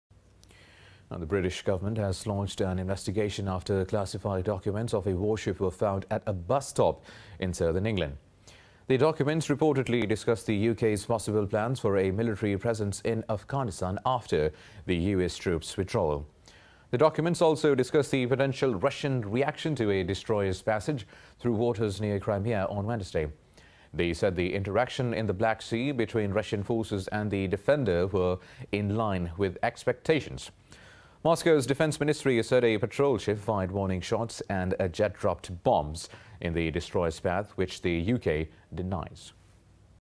Daily Top News | UK: CLASSIFIED DOCUMENTS FOUND AT BUS STOP | Indus News
The British government has launched an investigation after classified documents of a warship were found at a bus stop in southern England.